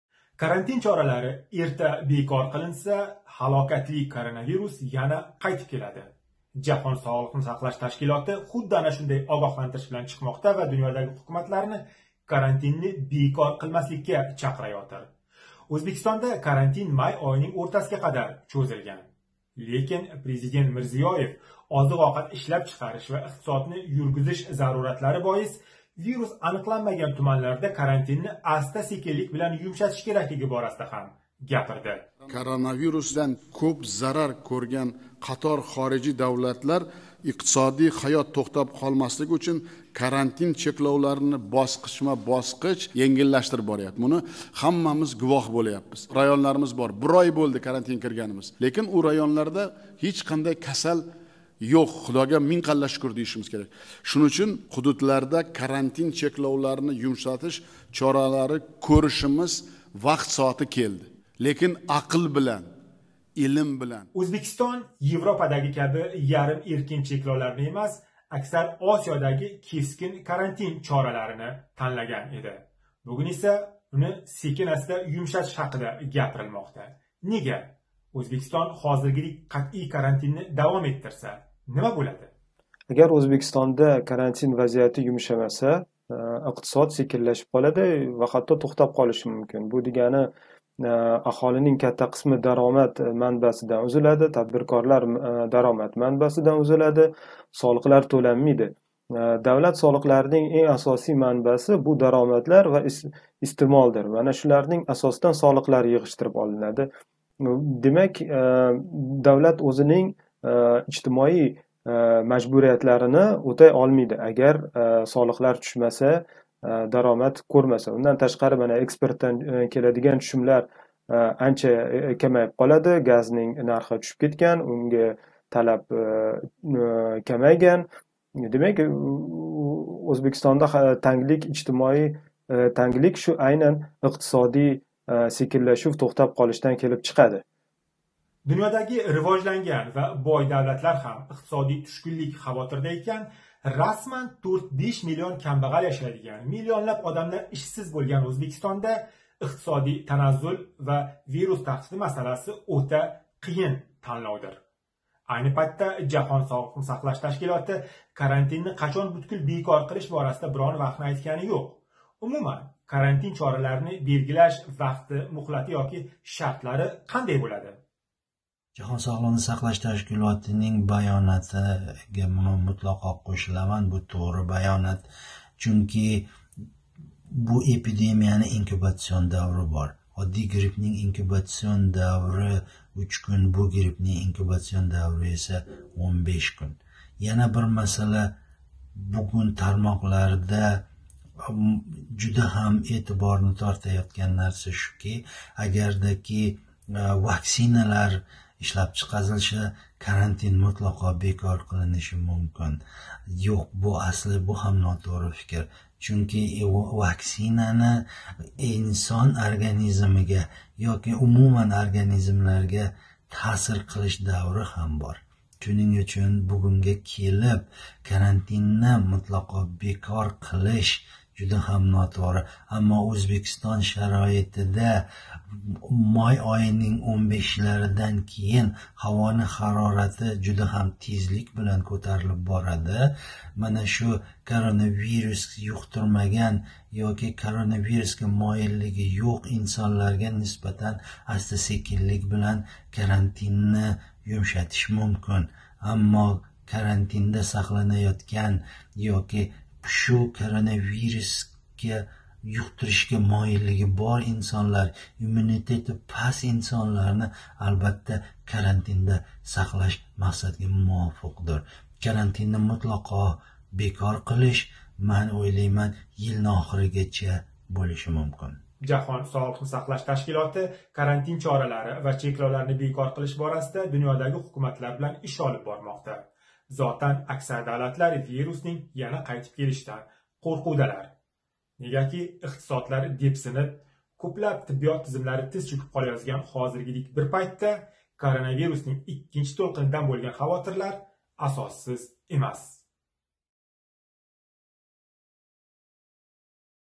Ўзбекистон карантинни юмшатишни бошлади. Коронавирус энди қайтмайдими?
Ўзбекистон карантин чораларини юмшатишни бошлади.

Президент  Мирзиёев бу борада эълон қилди.

 Сабаб - озиқ-овқат, ойликлар ва иқтисодиётни жонлантириш.

Вирус аниқланмаган ҳудудларда босқичма-босқич карантинни юмшатиш чоралари бошланган, баъзи соҳалар ишга тушаяпти.

 Ўзбекистон карантин чораларини 10 майга қадар узайтирганди.

ЖССТ эса олдинроқ дунёдаги ҳукуматларга огоҳлантириш билан чиққан ва карантинни эрта бекор қилиш, ҳалокатли коронавирус қайтиб келишига йўл очади, деб таъкидлаганди.

Ўзбекистоннинг ҳозирги  ҳудудма-ҳудуд карантинни бекор қилиб бориш амаллари ортидан вирус  қайтиши хавфи  кучаядими ёки йўқми?

Ўзбекистон бошлаётган ҳозирги енгиллатиш чоралари  босқичма-босқич карантиндан чиқишнинг илк палласими?

Ҳозирги карантинни юмшатиш чоралари Ўзбекистоннинг қайси ҳудудларида бошланди?

 Кимлар учун енгиллатимоқда? Сиз учун бу нимани англатади?

Би-би-си Ўзбек Хизмати журналисти Рустам Қобил мавзуни ўрганади.